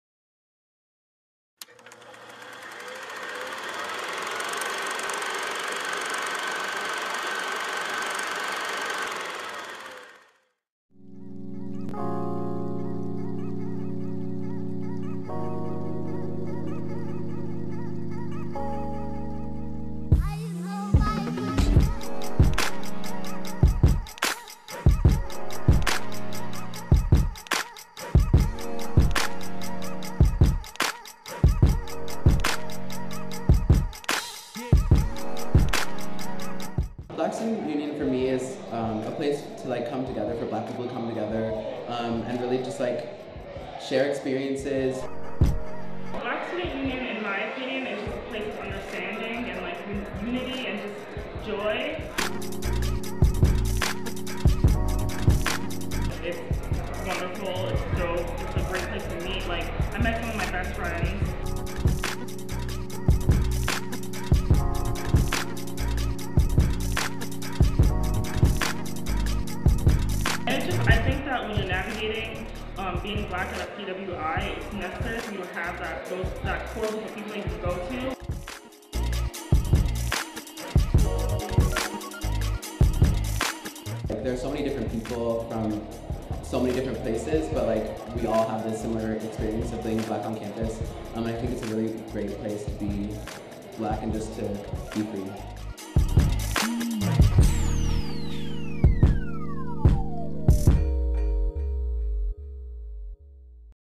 Black Student Union - Brown University
Black Student Union is a focal point for Black cultural, political and social activities.
Reel Features created this video. We make promotional videos for clubs at Brown.